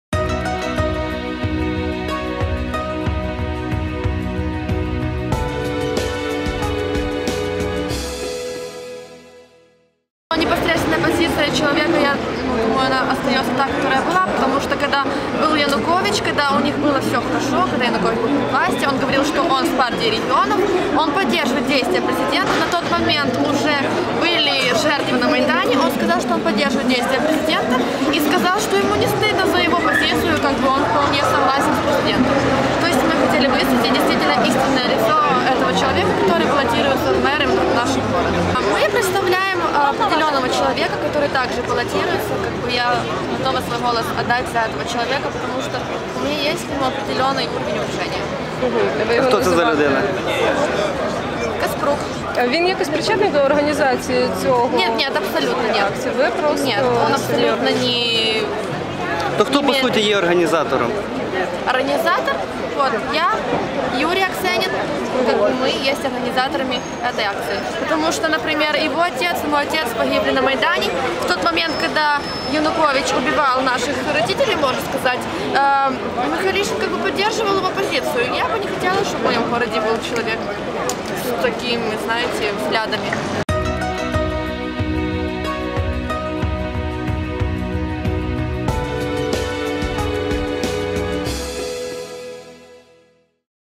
Ольга Щербанюк про Віталія Михайлішина
На сходах Чернівецької міської ради сьогодні, 21 травня, діти чернівецьких Героїв Небесної сотні закликали чернівчан не голосувати на виборах Чернівецького міського голови за Віталія Михайлішина. Юрій Аксенин та Ольга Щербанюк стверджують, що людина яка підтримувала Януковича, на чиїх руках кров сотень українців, в тому числі і їхніх батьків, не може керувати містом.